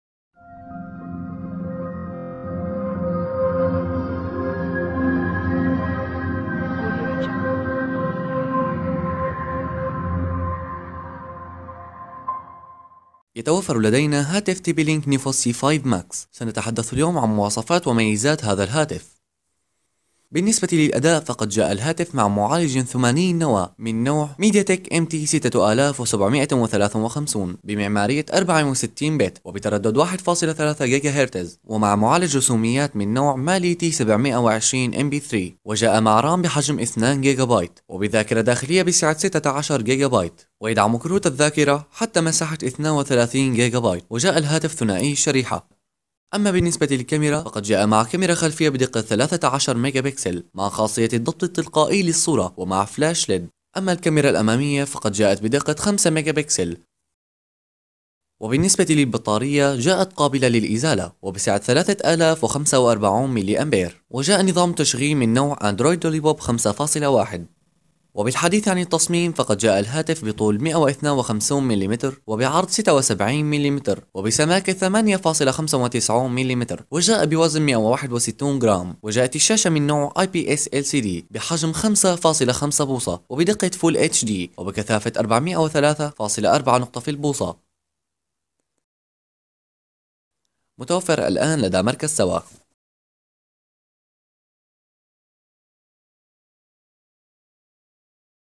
مراجعة هاتف neffos c5 max
مراجعة سريعة وشاملة لمواصفات هاتف تي بي لينك نيفوس سي 5 ماكس